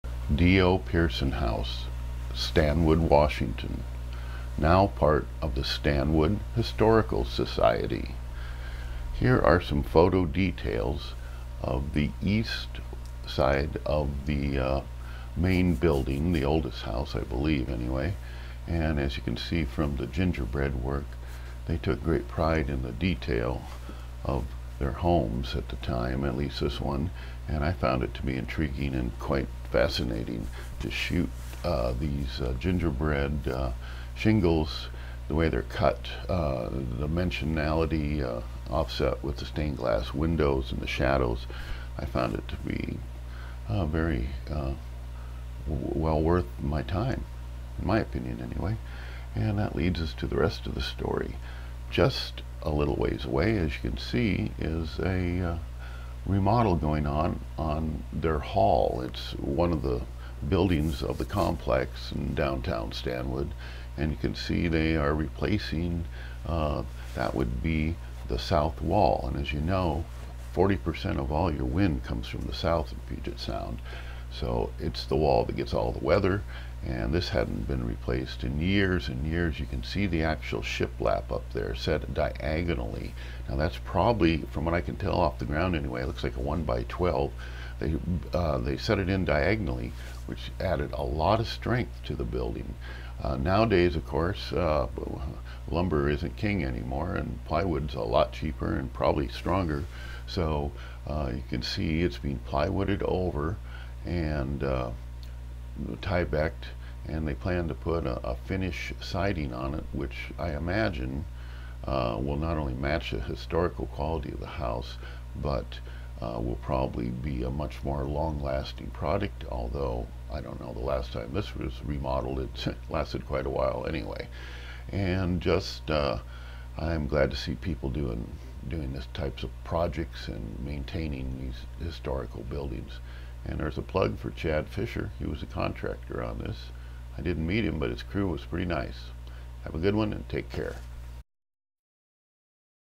photo tour # 91
Pearson historical house in Stanwood and the remodel of the hall.